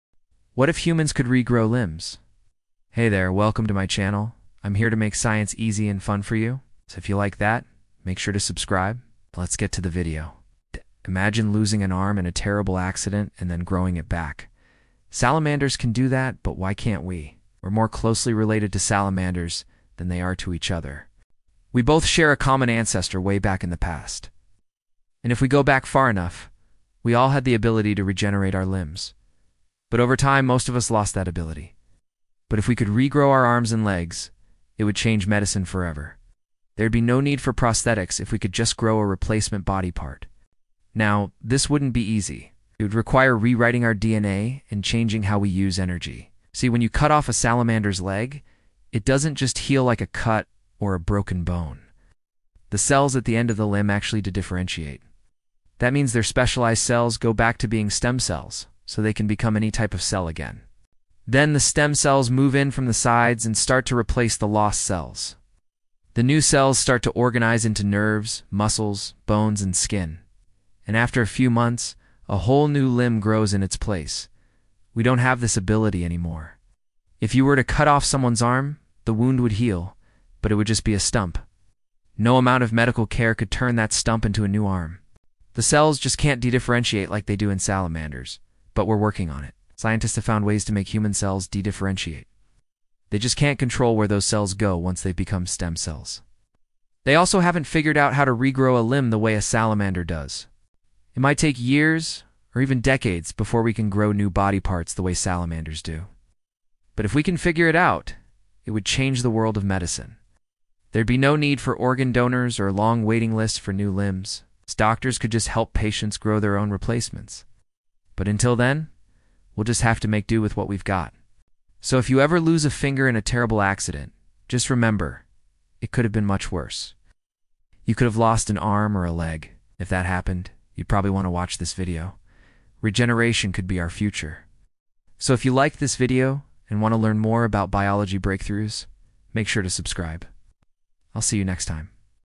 What If Humans Could Regrow Limbs
Exploring the possibility of human limb regeneration.